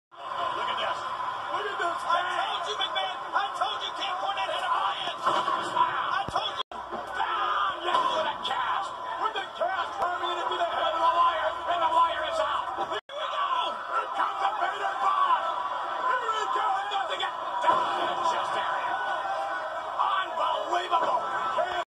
Camp Cornette Takes Out the Ultimate Warrior, 3 on 1 Situation - Vader, Owen Hart & British Bulldog
WWF WWE